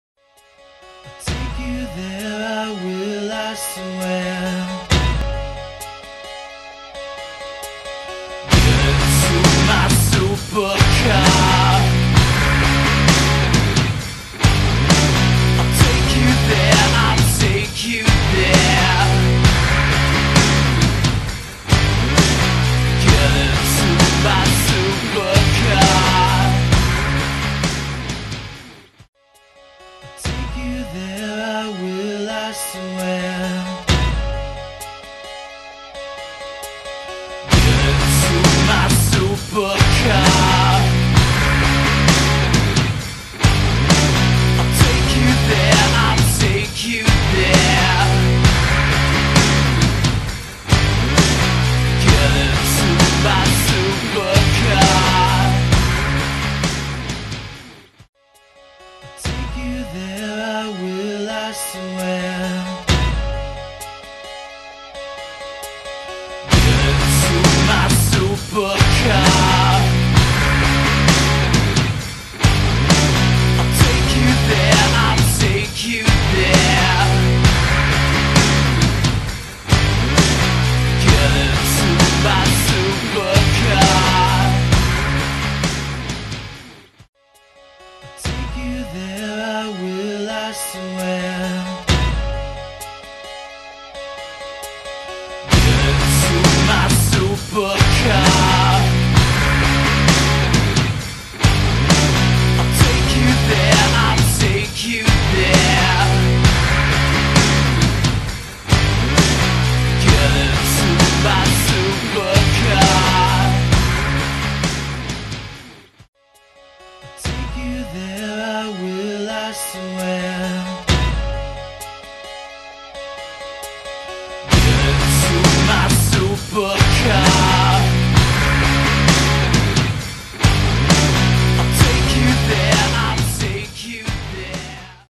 Flying car with foldable wings set to go on sale 2017
AeroMobil unveiled its flying car in Monaco on Thursday, and says it will be available for preorder this year The radical vehicle is set to cost between 1.2 million and 1.5 million euros ($1.3 million-$1.6 million) It has a driving range of about 100 kms (62 miles), but to use it in the air, you'll need a pilot's license